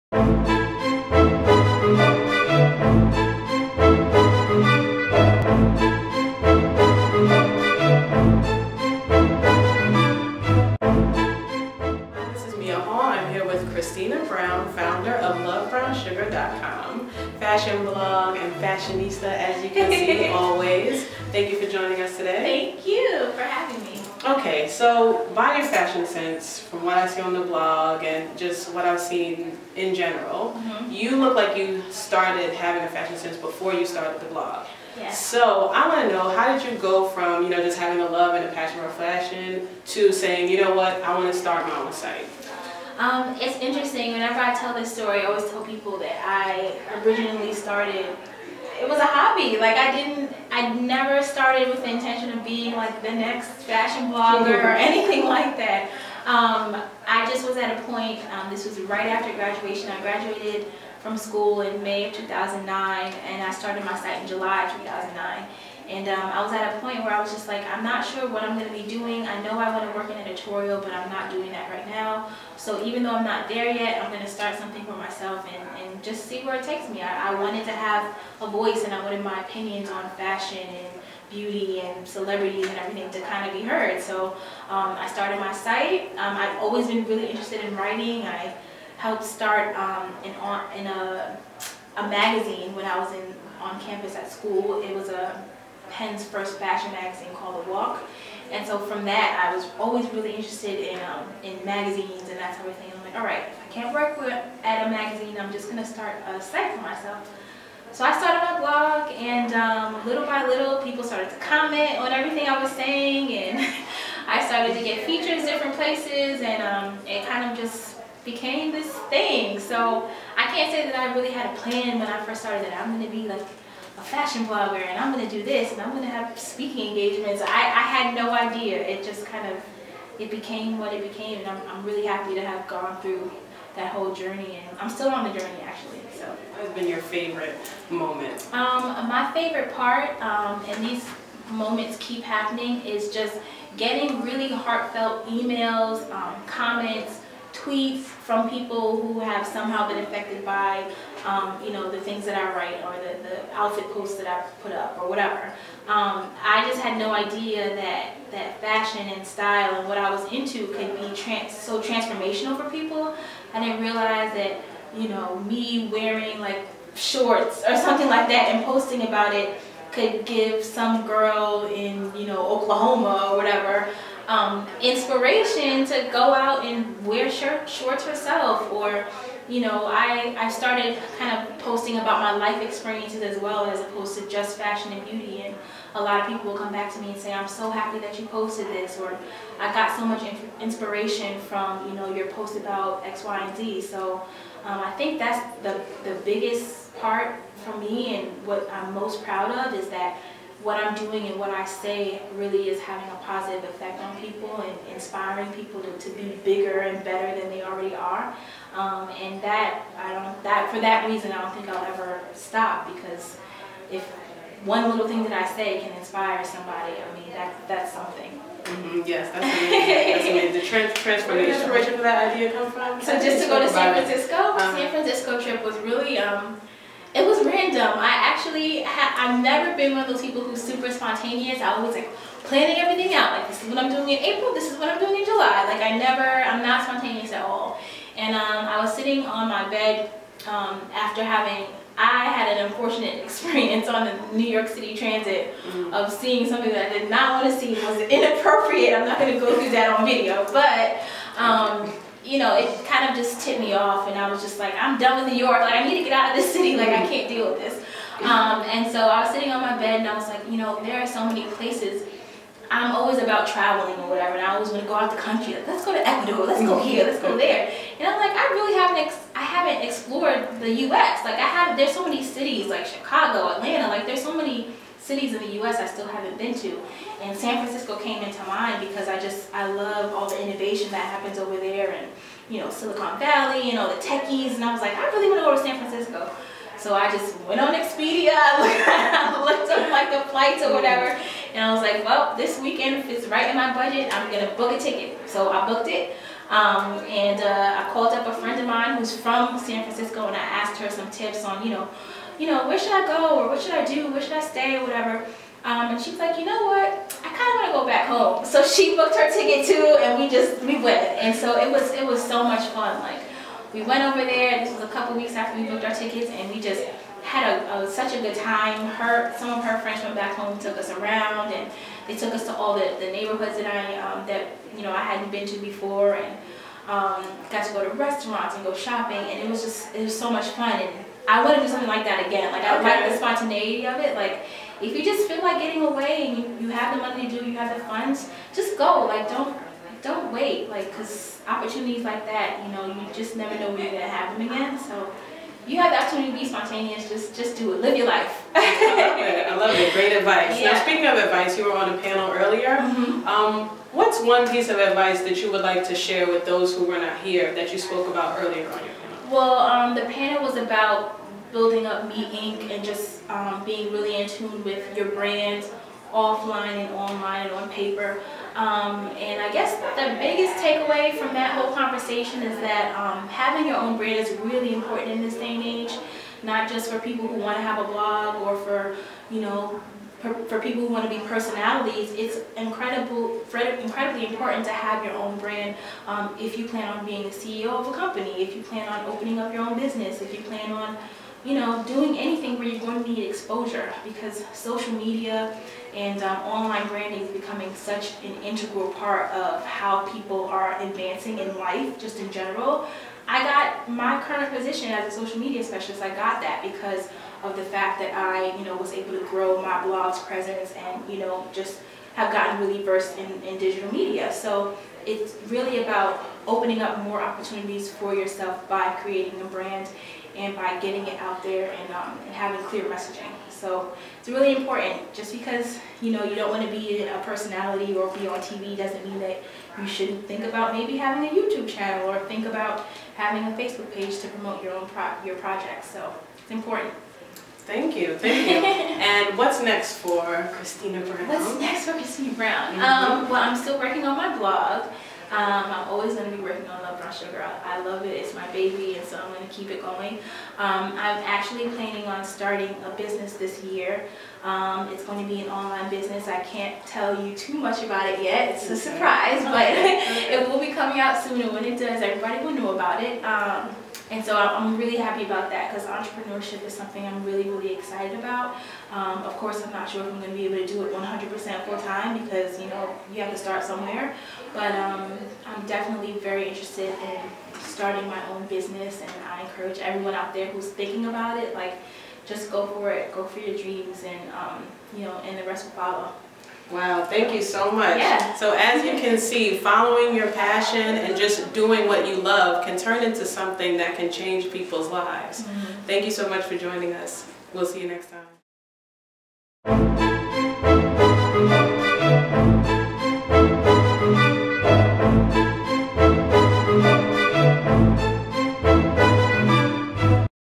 Rising Affluent Recap - How I Started My Blog with Love Brown Sugar
Mia Hall catches up with one of the panelists at the Rising Affluent Conference - fashionista and social media expert Christina S. Brown